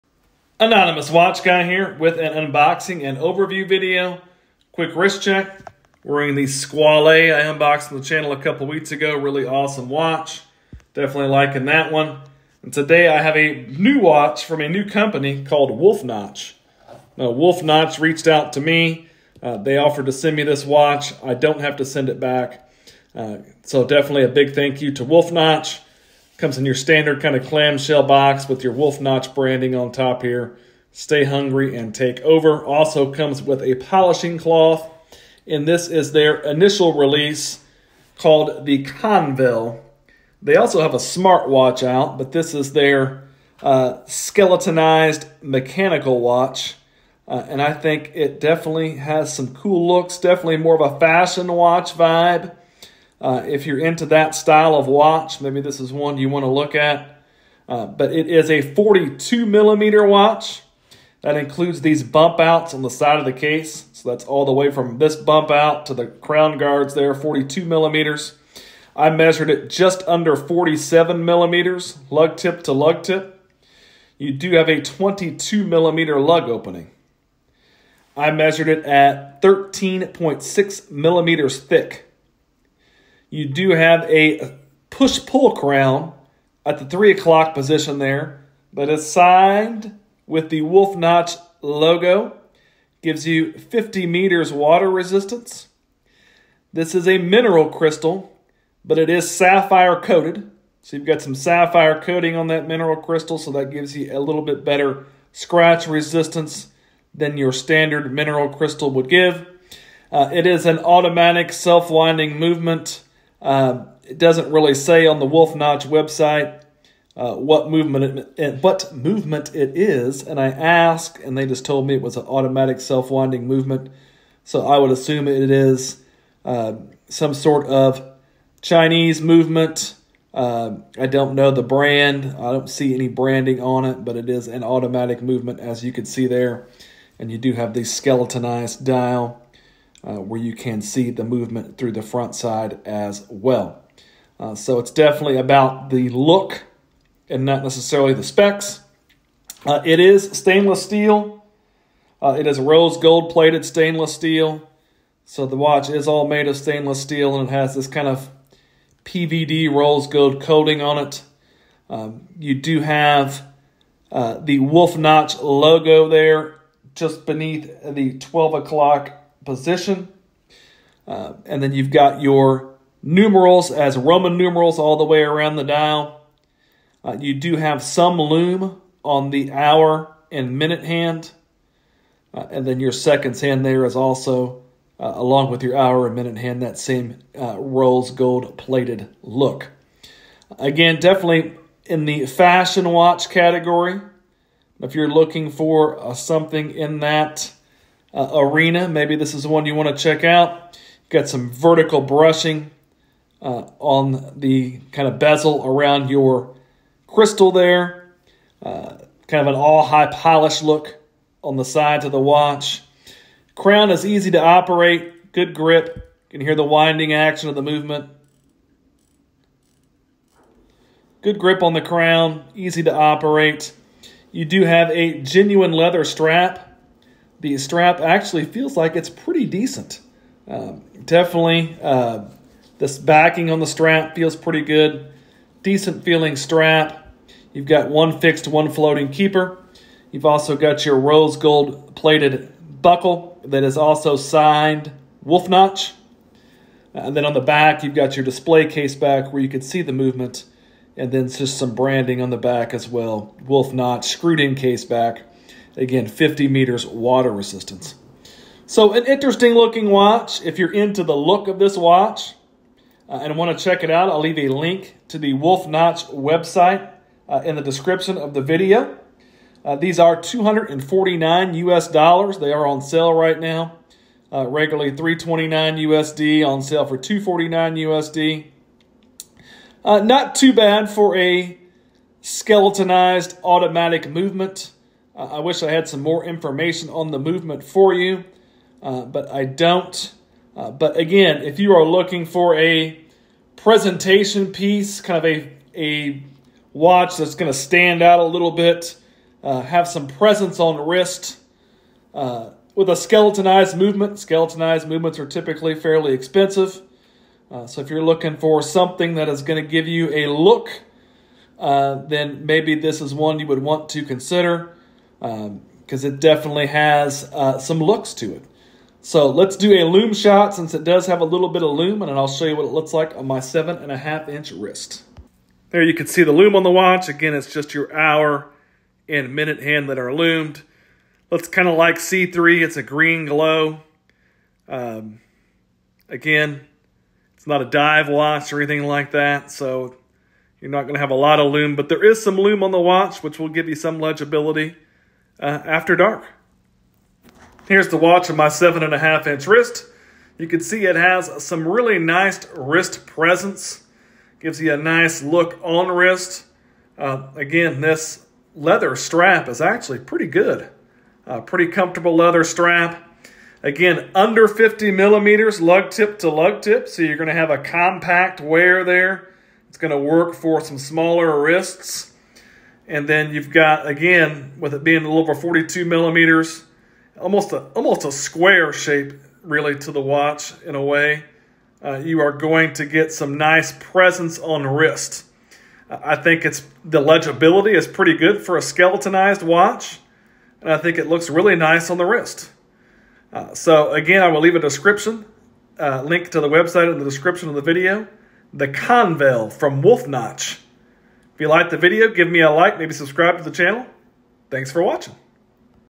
The Convel by Wolf Notch: Unboxing and Overview!
Taking a look at The Convel by Wolf Notch a rose gold plated skeletonized mechanical watch. Nice leather strap, solid wrist presence.